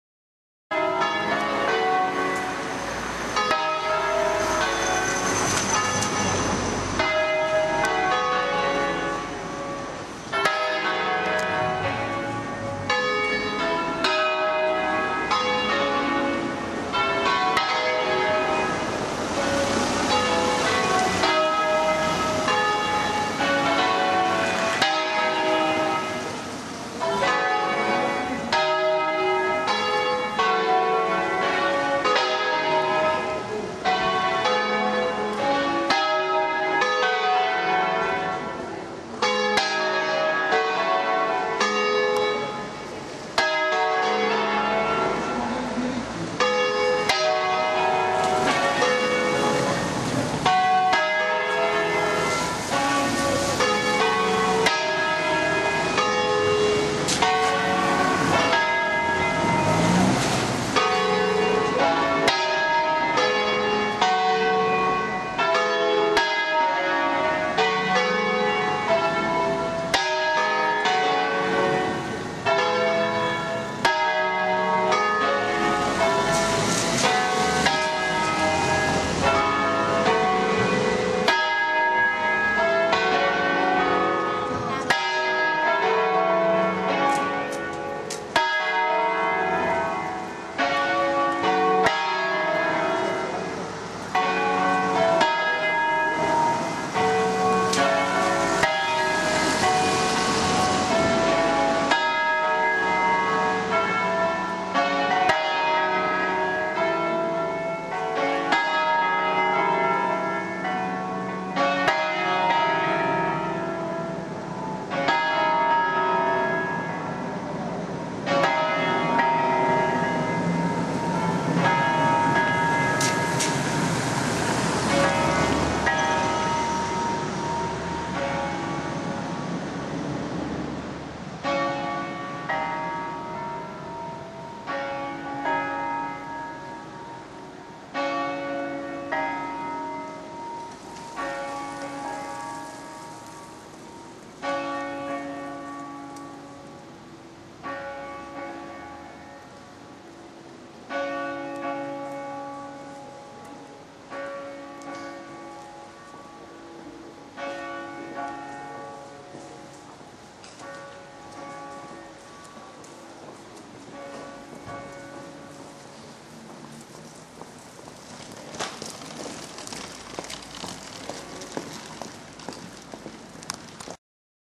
2015-02-15: Campane a distesa a Mede (PV)
5 Campane a distesa per la messa vespertina della domenica